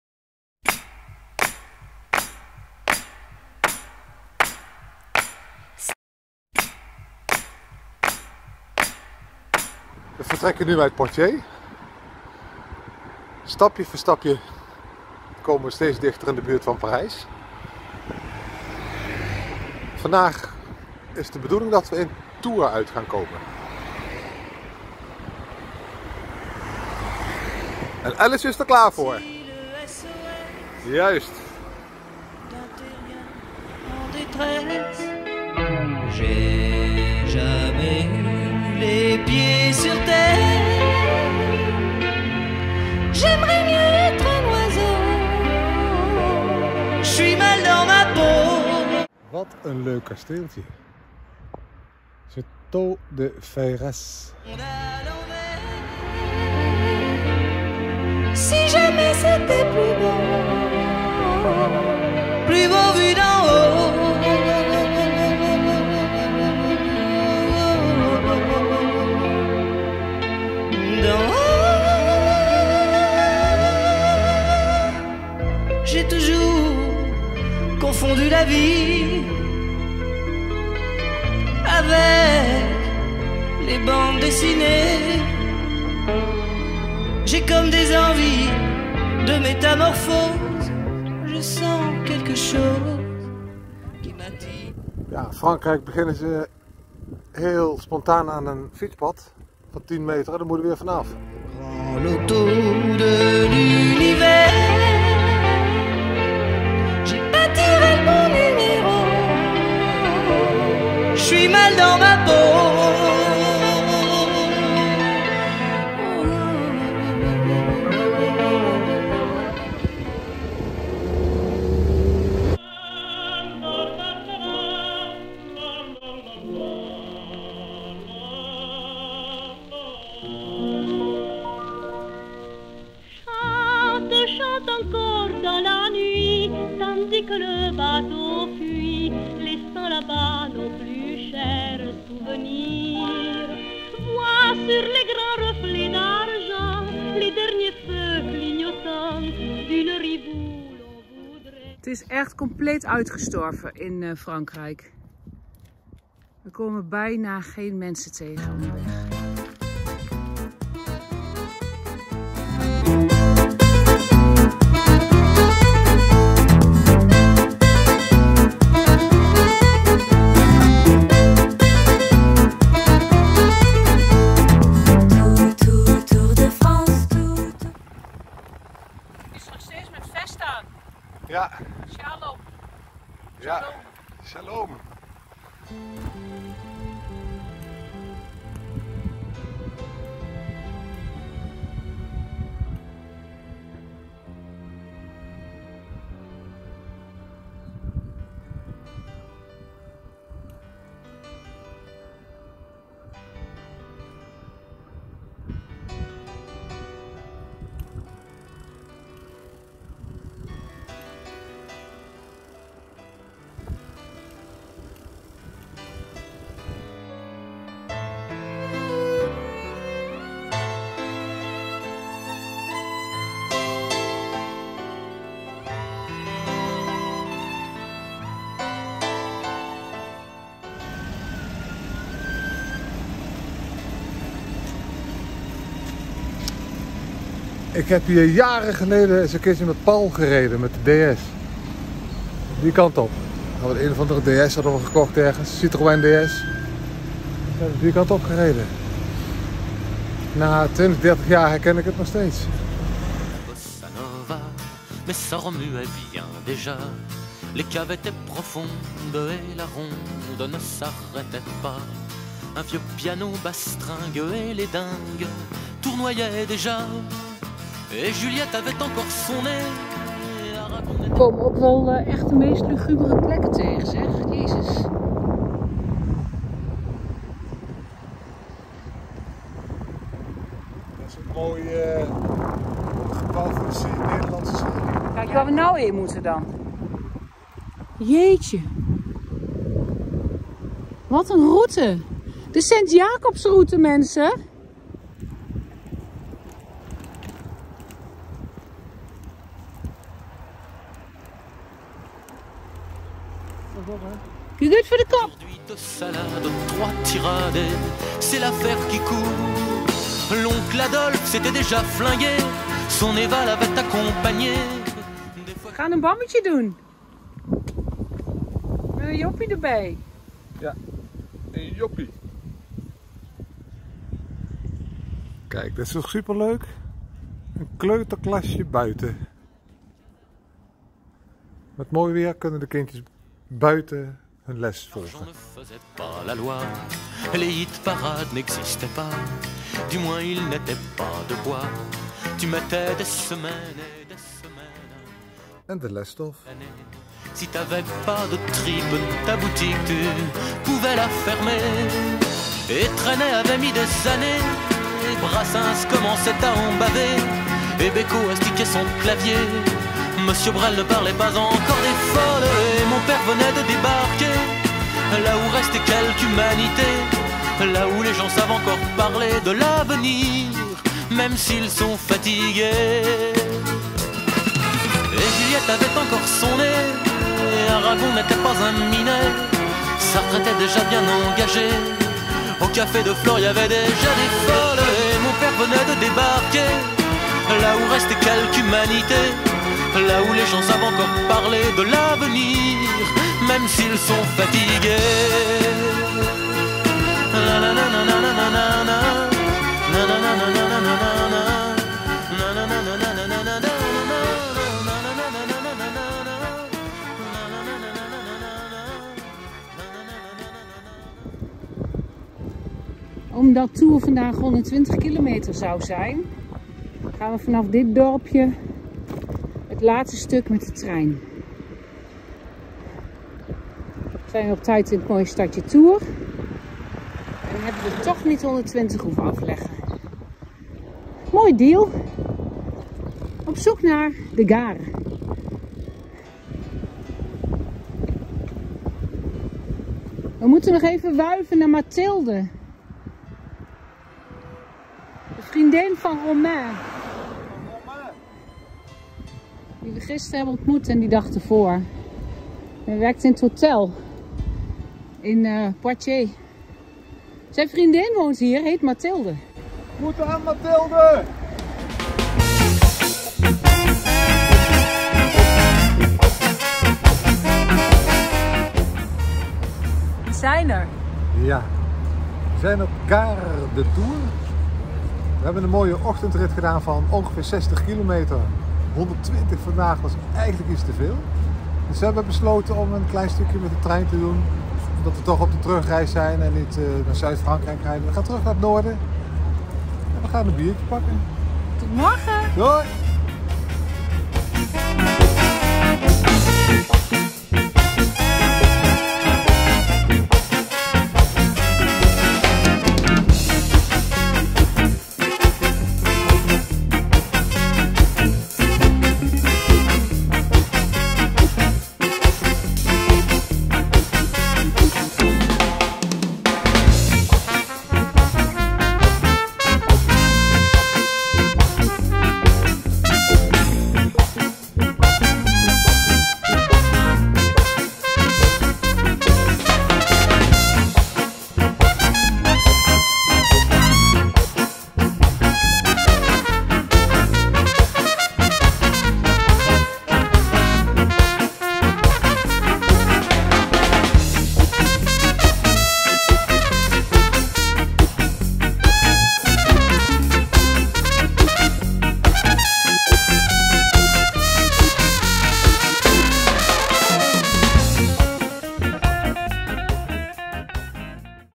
St Jacobs Route Dag 22 Poitiers l Tours
Fietsen St Jacobs Route  Dag 22 Poitiers l Tours

Cartoonist Ton Thimister
Muziek Roy Paci, les Poppys, Daniël Balavoine, Zazi, Nicolas Peyrac, Benny Neyman
Route Clemens Sweerman